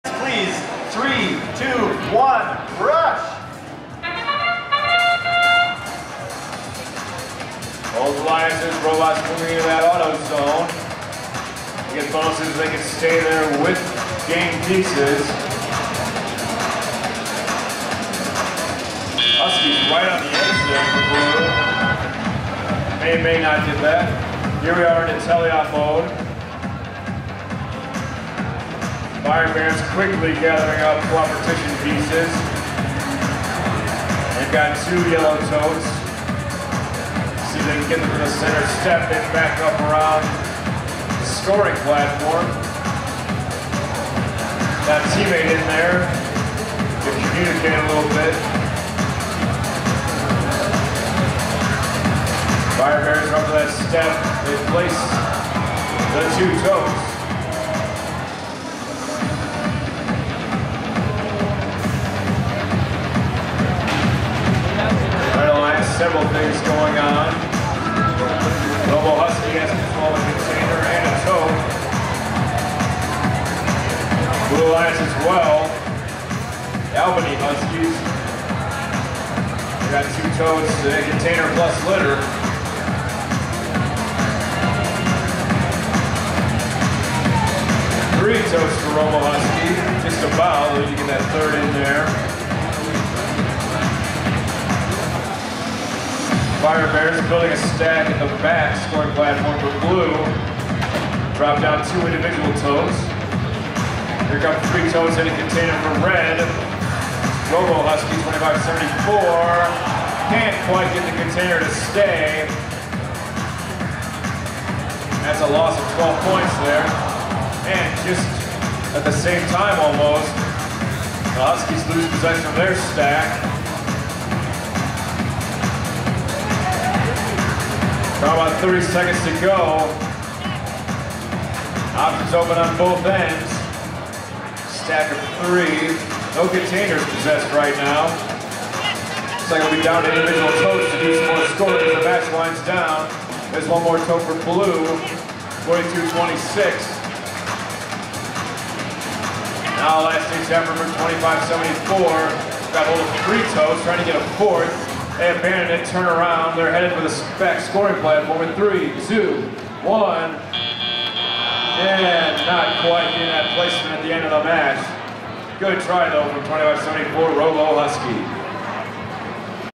Northern Lights Regional - Qualification 31
2846 in the qualification matches at the 2015 Duluth Northern Lights Regional that was on February 27-28